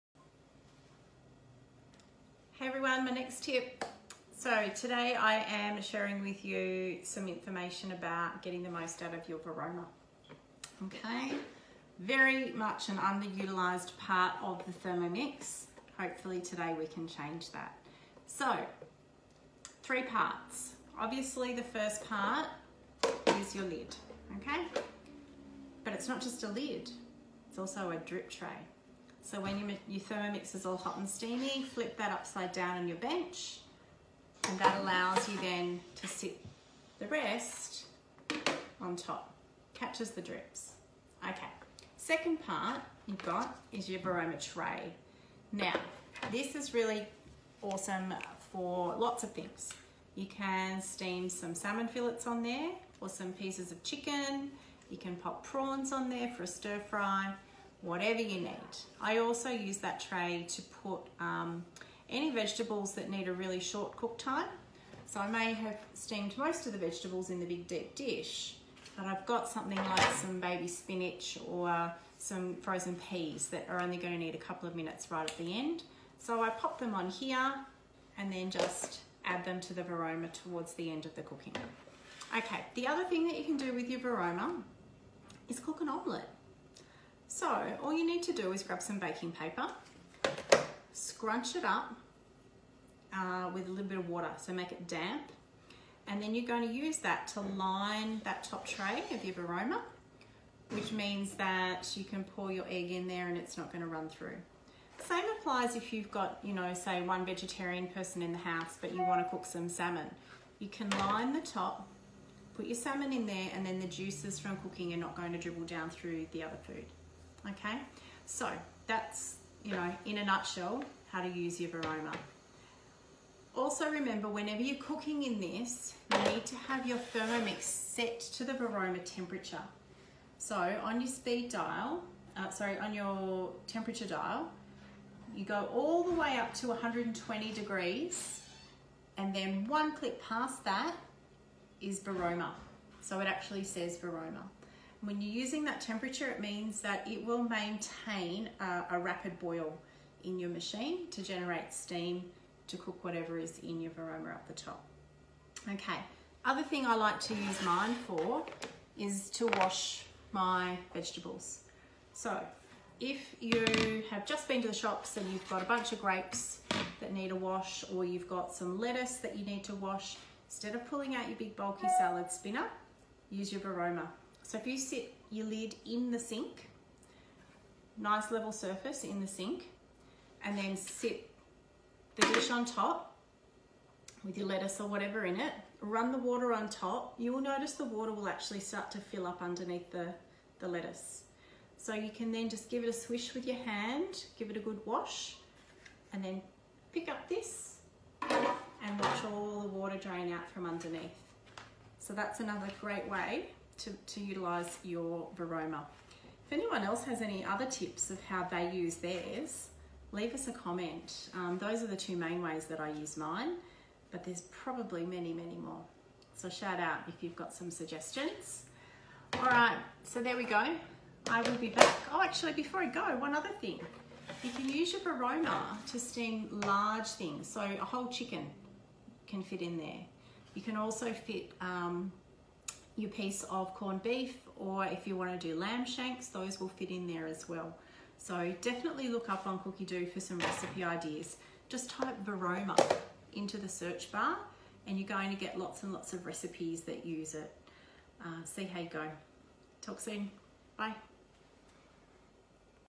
Cooking with your Thermomix Varoma
The Thermomix, when used to its full capacity, can hold up to 5.5L of food.
Let me show you how you can get the most out of your Thermomix by taking advantage of your steaming attachment... the Varoma!